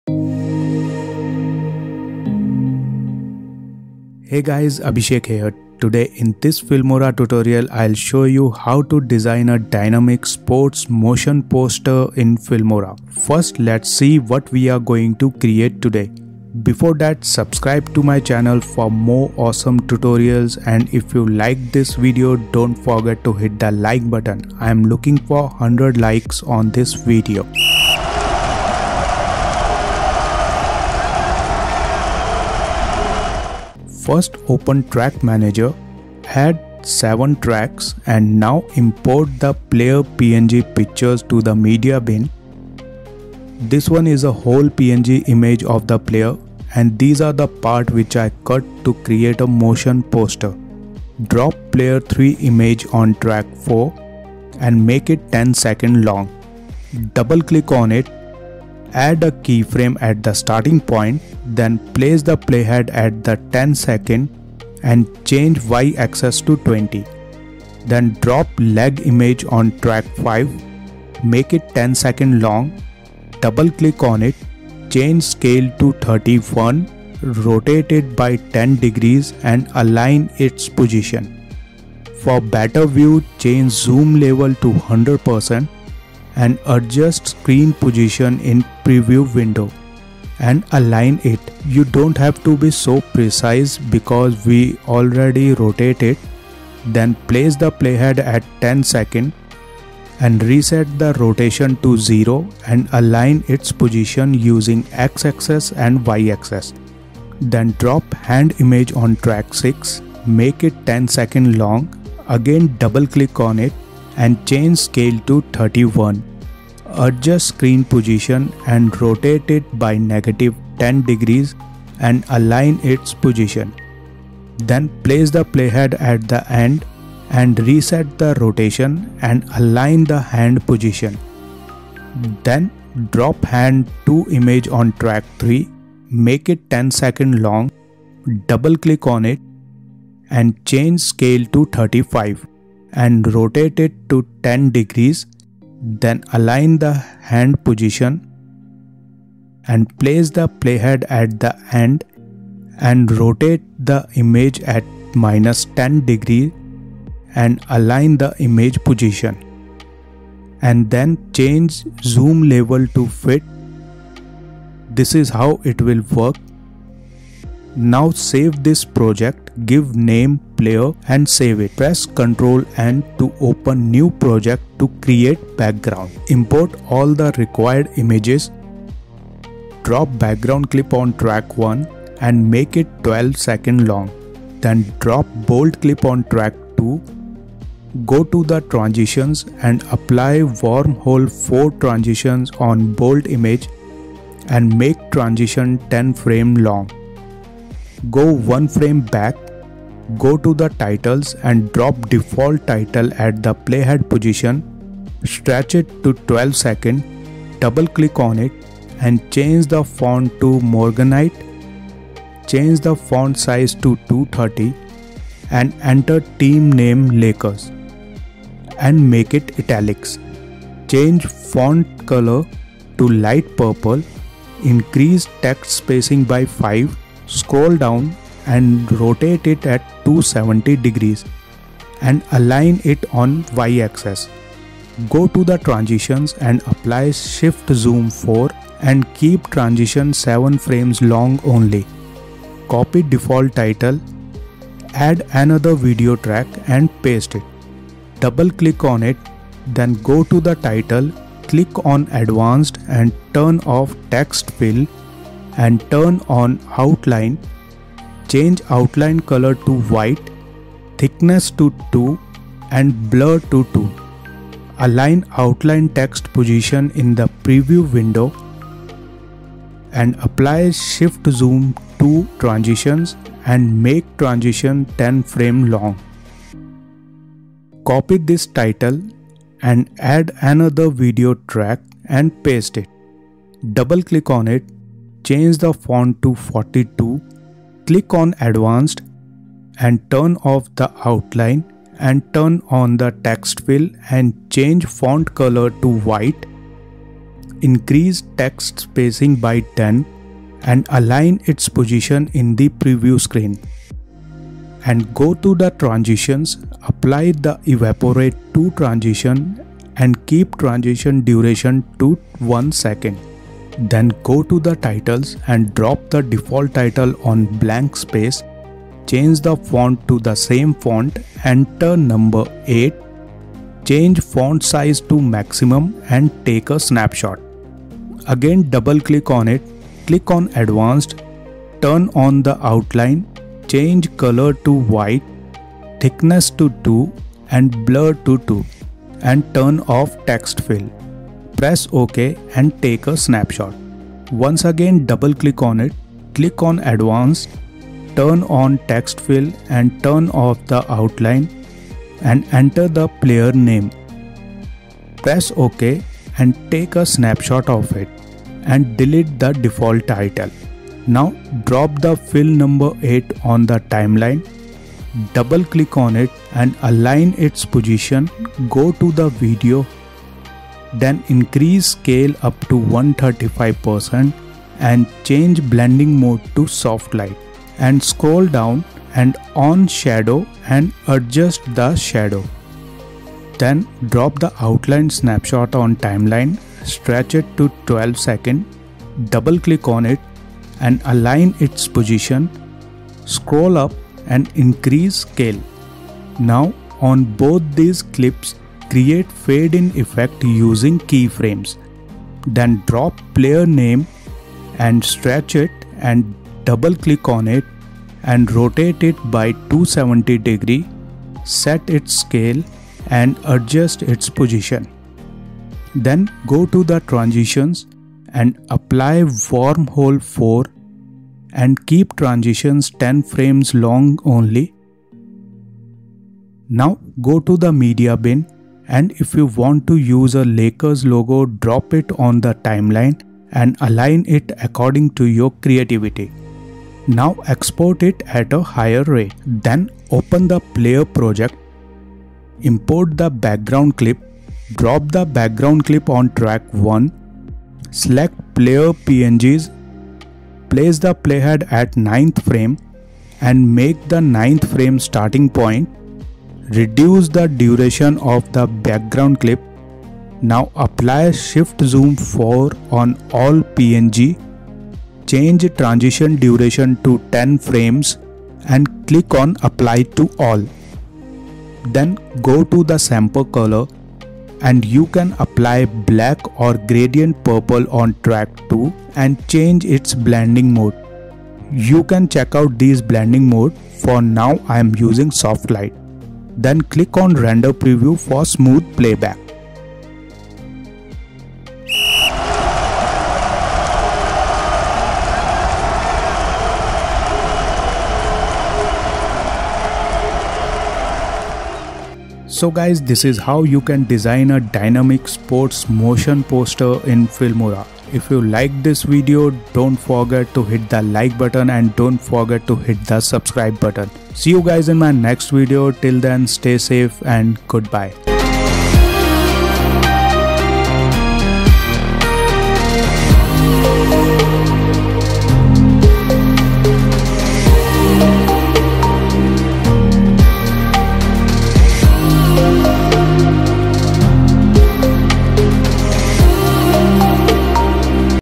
How To Design Motion Poster | How to Create Motion Poster in Filmora - step by step
Hey Guys, Abhishek here In this filmora tutorial video,  I am going to show you how to design motion poster in filmora. In this video, we are going to design a sports motion poster in filmora.

Support me
▷ Become a member to show your support! 🔔